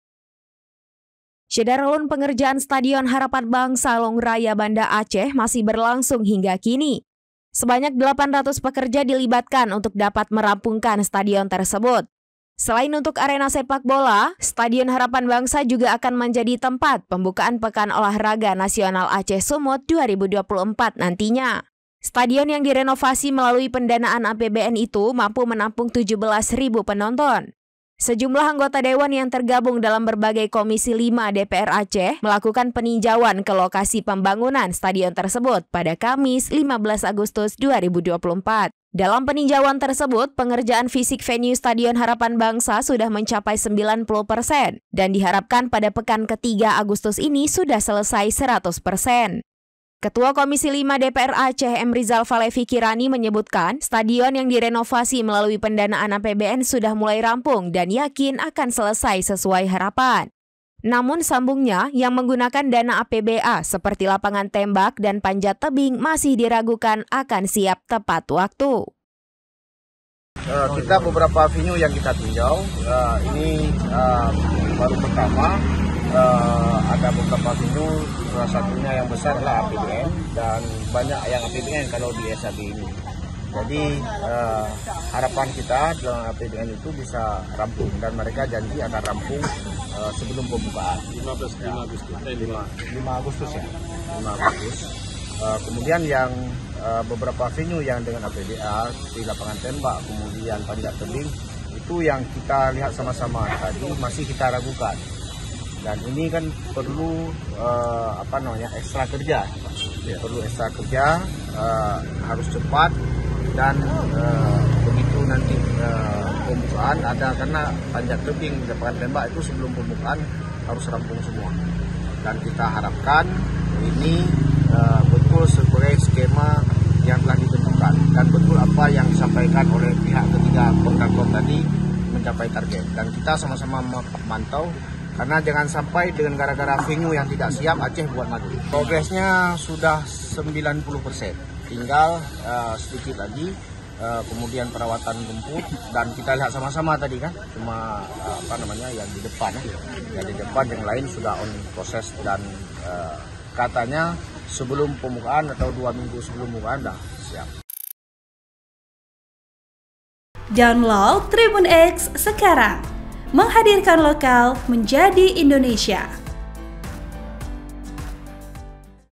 DPR Aceh Ragukan Pembangunan Venue PON yang menggunakan APBA Siap Tepat Waktu
SERAMBINEWS.COM-  Pengerjaan Stadion Harapan Bangsa, Lhong Raya, Banda Aceh masih berlangsung hingga kini.

Sebanyak 800 pekerja dilibatkan untuk dapat merampungkan stadion tersebut.

Selain untuk arena sepak bola, Stadion Harapan Bangsa juga akan menjadi tempat pembukaan Pekan Olahraga Nasional (PON) Aceh - Sumut 2024 nantinya.

vo: dara
ev: nash
laporan: hendri